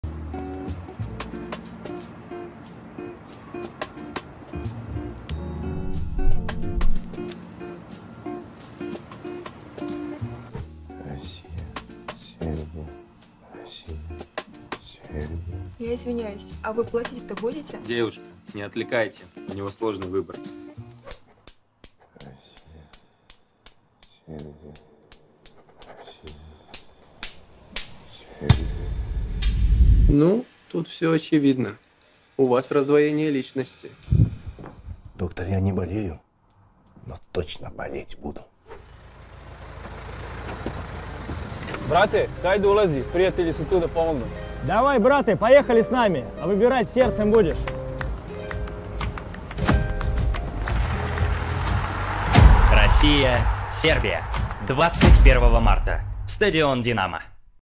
Самый сложный выбор в жизни Биковича | Промо-ролик к матчу Россия – Сербия
Товарищеский матч Россия – Сербия – это игра братских народов. Болельщикам будет непросто определиться, за кого болеть в этом матче. Но самый сложный выбор у Милоша Биковича.

Звезда «Холопа» – гражданин и Сербии, и России. Это усложняет принятие решения, но ему на помощь приходят Федор Чалов, Милан Гайич, Федор Кудряшов и Дмитрий Сычев.

🇷🇺 или 🇷🇸? Како се одлучити за кога навијати? Најтежа животна одлука Милоша Биковића. 😅

Братска утакмица између Русије и Србије – ускоро!